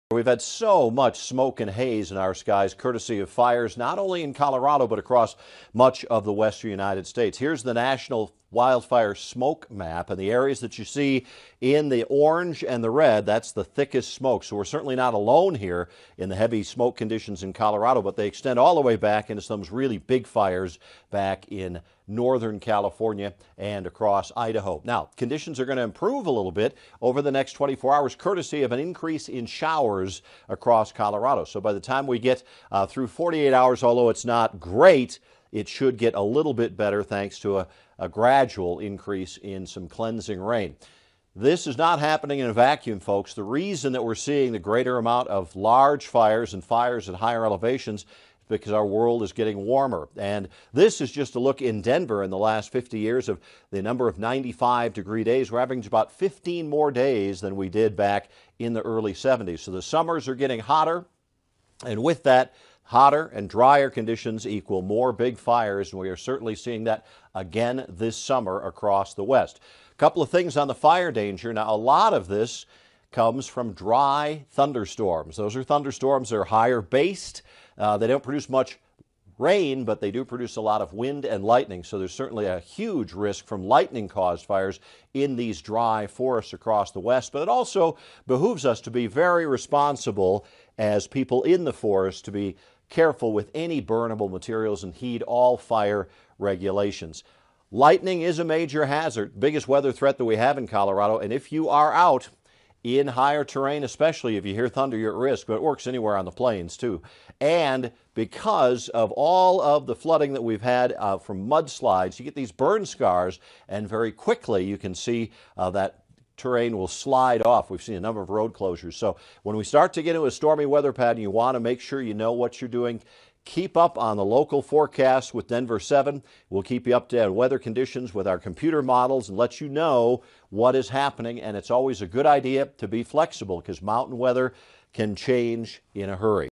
Mike Nelson's Tuesday afternoon wildfire smoke forecast
Chief Meteorologist Mike Nelson takes a look at the upcoming smoke forecast this week from wildfires burning across the West.